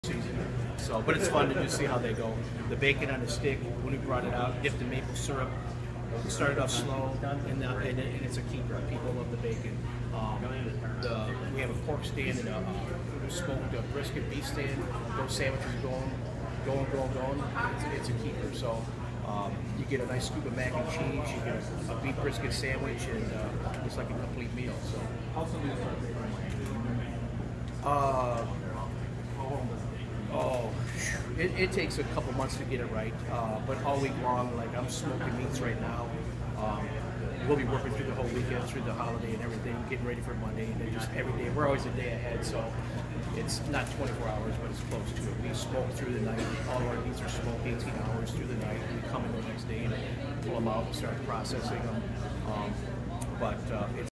Executive Chef Szubeczak talks bacon and 18-hour smoked meat
The Comerica Park kitchen has also been busy preparing for the big day. &quot;Like all week long,&quot; said Executive Chef Mark Szubeczak. &quot;I&apos;m smoking meats right now. We&apos;ll be working through the whole weekend and the holiday getti